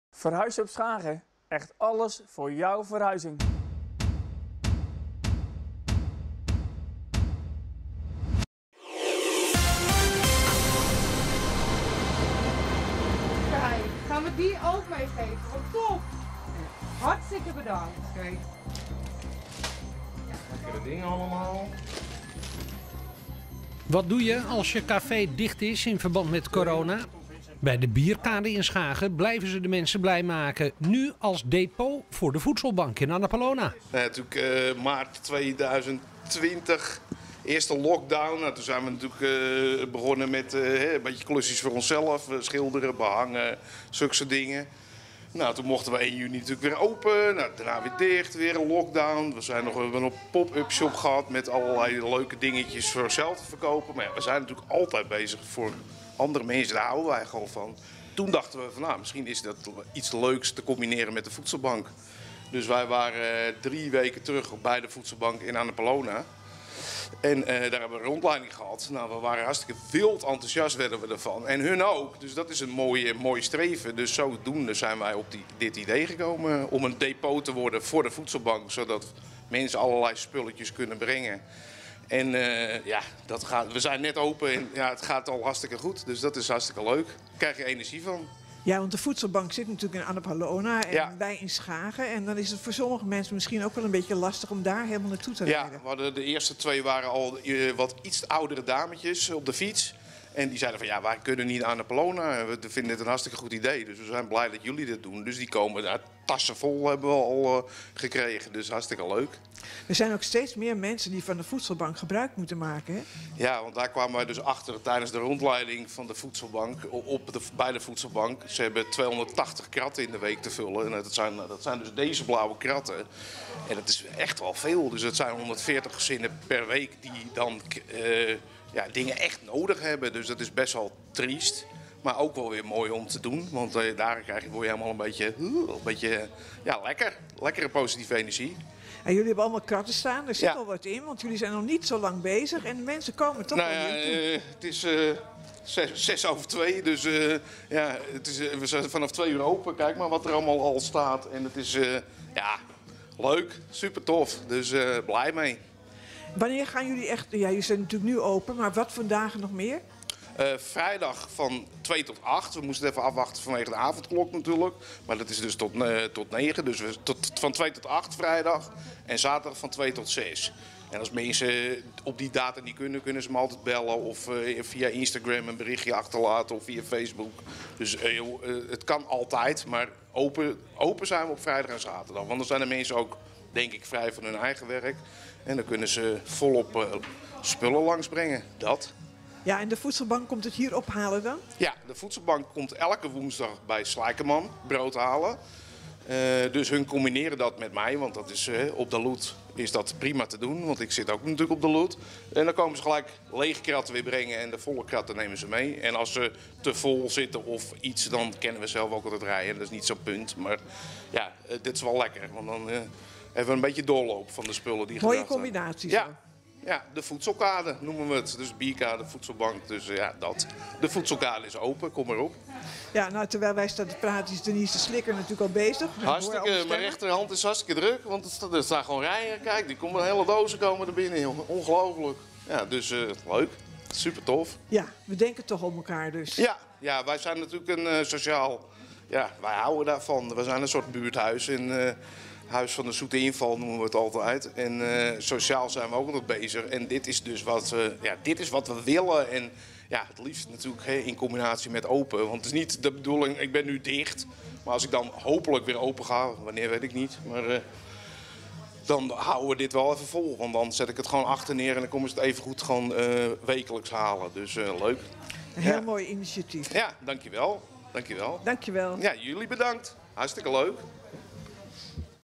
Cafe De Bierkade is depot voor de Voedselbank
“Voor ons allemaal zijn het moeilijke tijden en daarom denken wij nu nog meer aan mensen die het ook moeilijk hebben”,  zegt Denise Slikker van De Bierkade in Schagen.

Een verslag van Kitty Mertz en Ed van Oerle